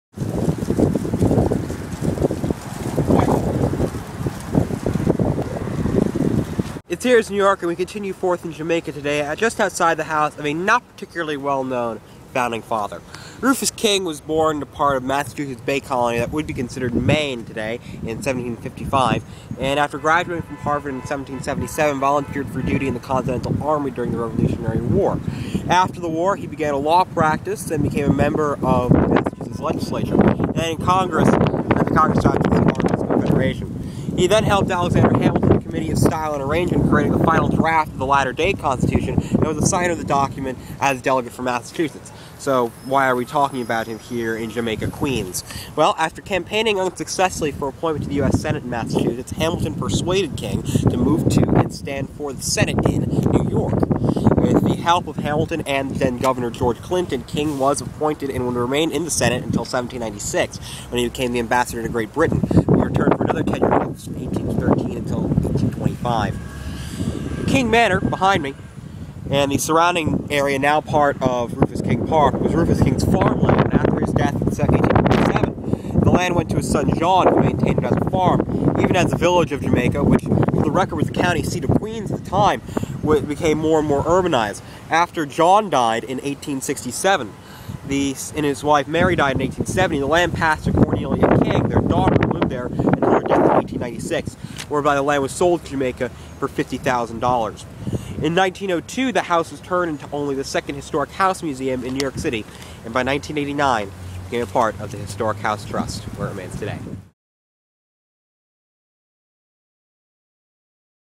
Here Is New York 004: King Manor
Daniel discusses the life of Founding Father Rufus King and his house in Jamaica, King Manor.